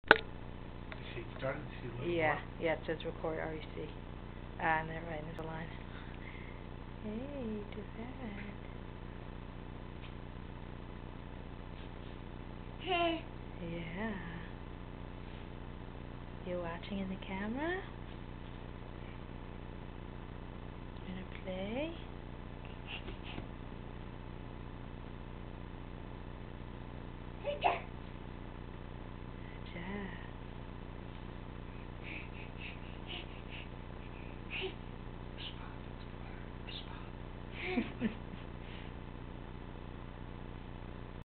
tiferet and explorer.MOV
Here is Tiferet playing with Explorer.